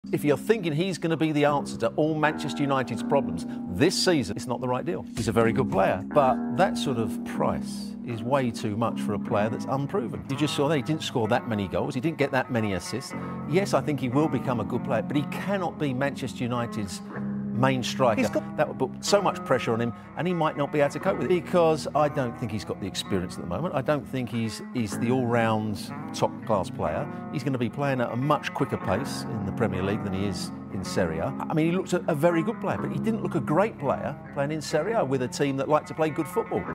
Stewart Robson Says Højlund Shouldn’t Start For Man Utd
Stewart Robson Says Rasmus Højlund Shouldn’t Start For Man Utd